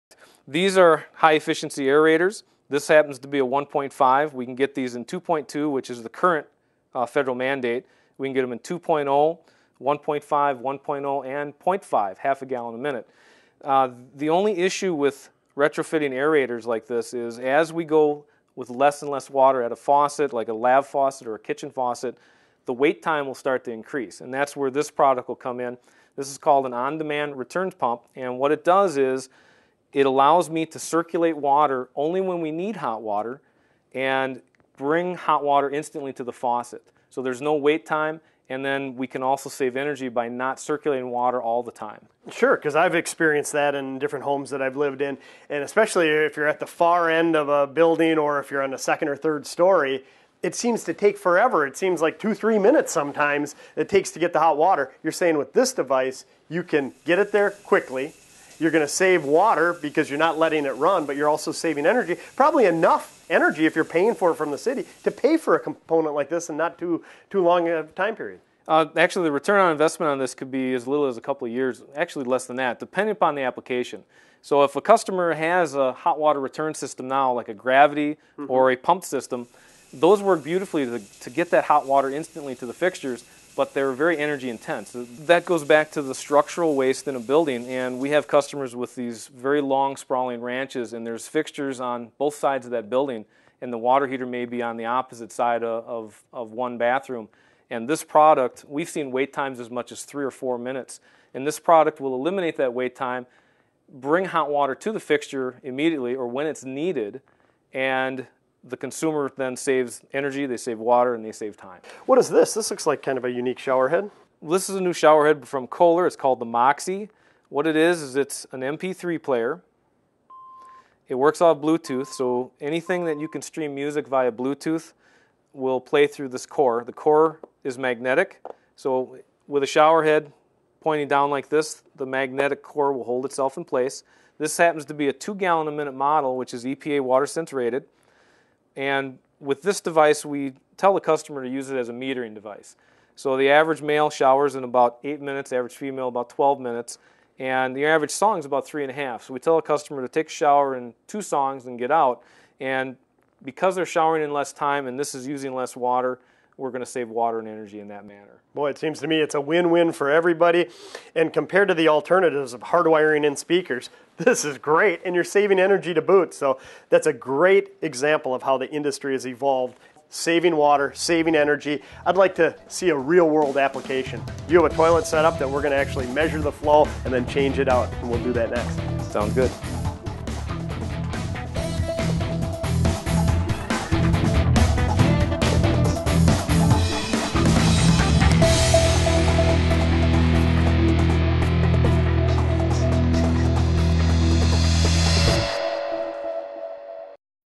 High Efficiency Faucet Aerators, On Demand Return Pump, Moxie Shower Head Save Water & Money!
EPA WaterSense products offer energy-saving benefits, plus high performance!  Here's a quick look at high efficiency faucet aerators, on-deman return pumps and Kohler's Moxie MP3 Shower Head.  Master Plumber Justin Castleman takes us through some of their features and benefits, including improved performance and saving water!  Courtesy to Crew/Cut Teleproductions for their permission to use this video.